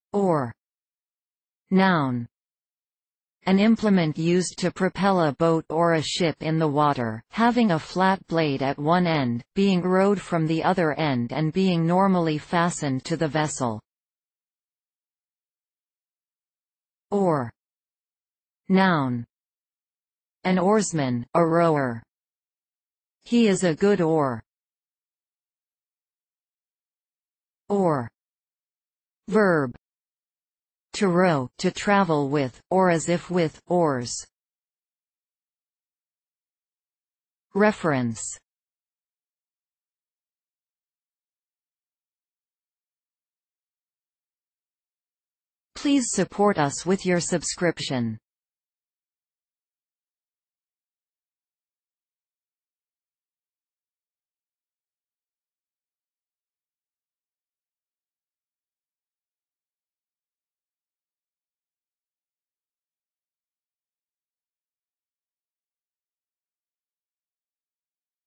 Oar | Definition of oar
See here, the definitions of the word oar, as video and text.

oar (noun)
    An implement used to propel a boat or a ship in the water, having a flat blade at one end, being rowed from the other end and being normally fastened to the vessel.

oar (noun)
    An oarsman; a rower.
        He is a good oar.

oar (verb)
    To row; to travel with, or as if with, oars.

Reference:
    oar